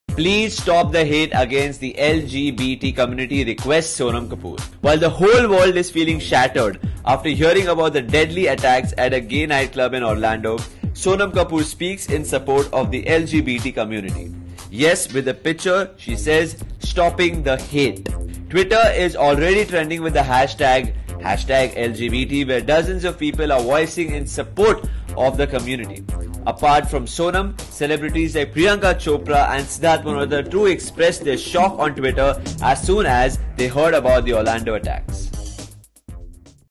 Please STOP the hate against the LGBT community, requests Sonam Kapoor
While the whole world is feeling shattered after hearing about the deadly attacks at a gay nightclub in Orlando. Sonam Kapoor speaks in support of the LGBT community. Yes! With a picture that says, “Stopping The Hate”, Sonam just expressed her grief on Instagram saying, “Please please stop the hate… “It was times like these when I thought my father, who hated guns and had never been to any wars, was the bravest man who ever lived”. ~Harper Lee, To Kill a Mockingbird, Chapter 11. orlandoshooting tolerate love peace lgbt allreligionsleadtoonegod.” Twitter is already trending the hashtag LGBT where dozens of people are voicing in support of the community. Why so much hate against them? After all, they too are humans right? Why kill them? Why torture them for being alive? The answers lie within us but turns out the world is far from abiding by humanity and peace of living.

Apart from Sonam, celebs like Priyanka Chopra and Sidharth Malhotra too expressed their shock on Twitter as soon as they about the Orlando attacks. While PC wrote, “Heartbreaking to hear about the OrlandoShooting May peace come upon this earth.. Prayers for the victims and their families.prayerforlove. Sid seconded similar thoughts stating, “So Sad to hear about the massacre in Orlando here My prayers n condolences to the families who lost their loved ones,stop this violence.”